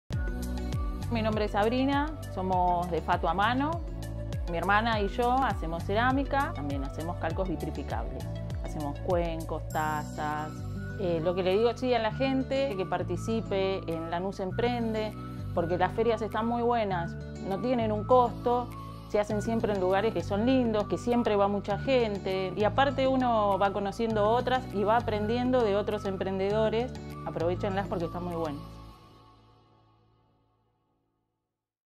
ORGULLOLANUSENSE 📣
✨ En esta oportunidad, visitamos a Sabrina, que junto a su hermana Anabella ya hace un año que son parte de las ferias de @lanusemprende con @fatto.a.mano.2020.

Es un emprendimiento de cerámica y calcos vetrificables, donde ellas llevan a cabo todo el proceso incluso de pintado y horneado.

¡Queremos mostrar todo el talento de nuestros vecinos y apoyarlos en su crecimiento! 💪